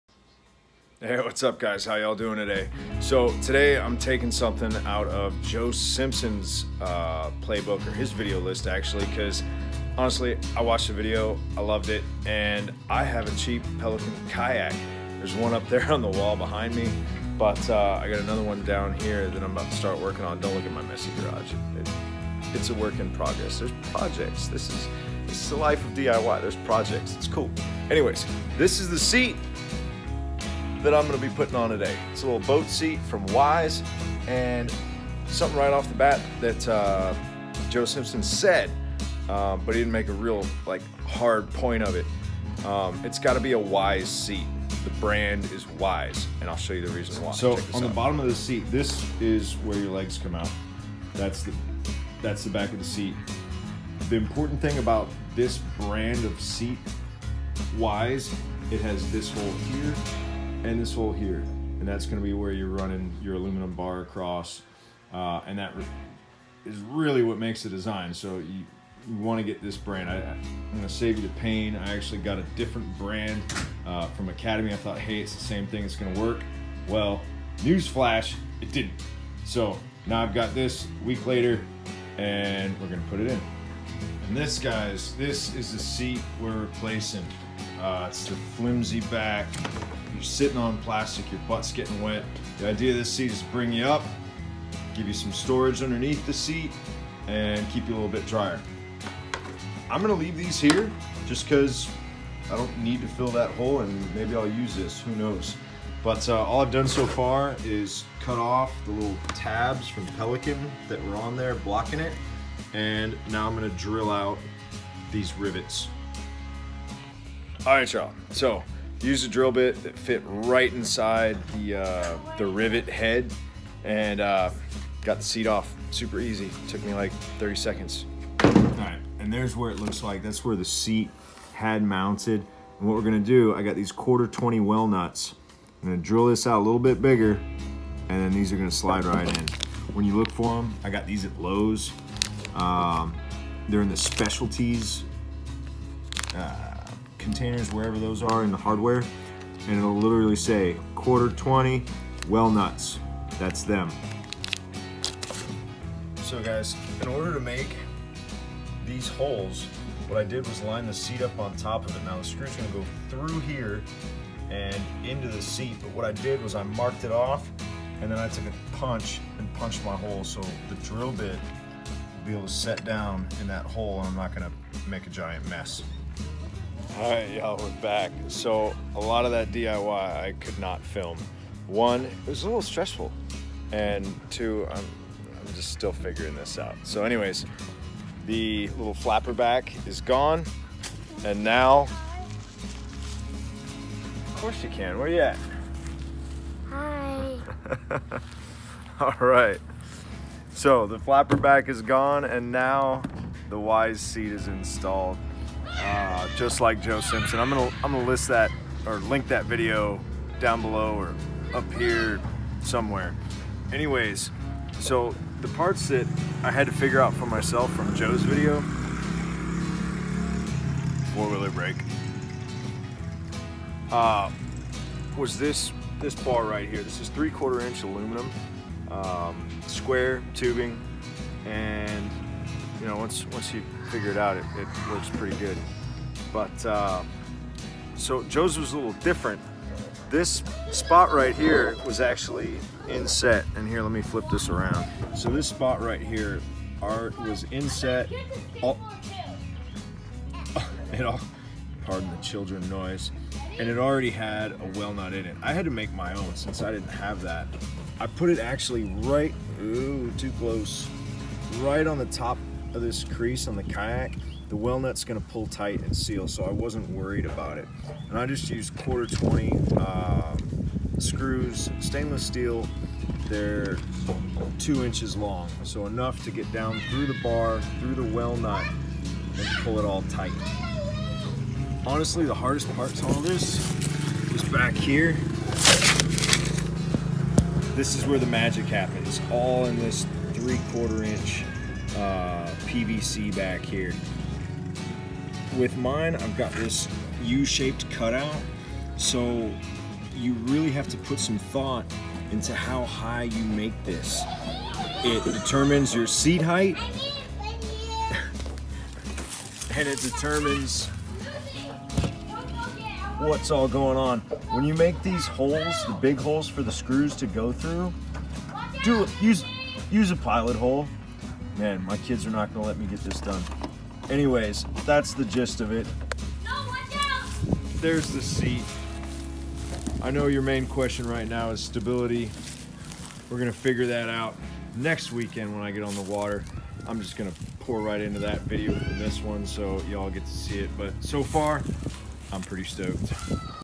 Kayak Seat UPGRADE DIY, Pelican Bandit 100 Cheap kayak seat upgrade to enhance your day
Hey guys, I saw a video Joe Simpson did and loved his kayak seat upgrade, had to try it out on my little pelican bandit and show y’all. I love it. DO this if you're Fishing from a Kayak like this!!!